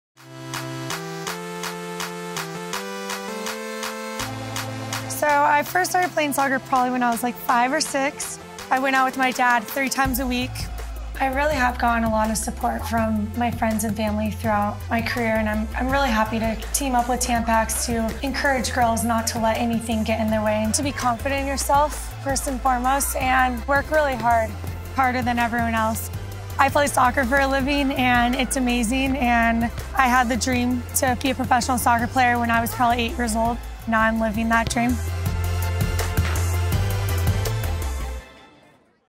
Take a Look at Alex Morgan’s Journey to the Top
Check out exclusive behind-the-scenes content showcasing Alex Morgan’s journey to the top, presented by Tampax Pearl Active. From setting goals to getting support to playing with confidence at every age, Alex shares how hard work and determination has been a part of where she is today . Follow @Tampax online and use AwesomelyActive to join the conversation.